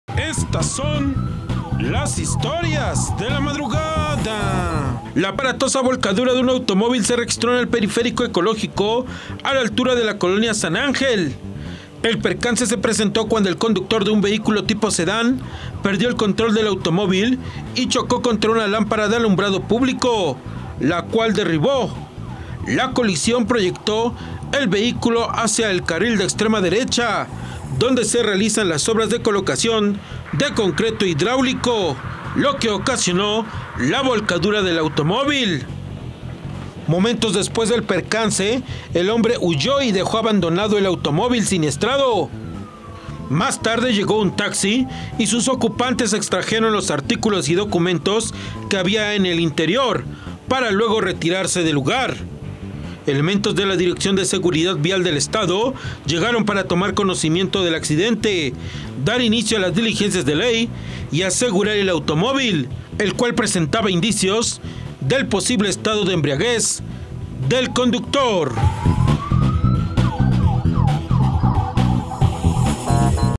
Historias de la madrugada 20 junio
🚗💥 Tras chocar contra luminaria se vuelca en el periférico ecológico.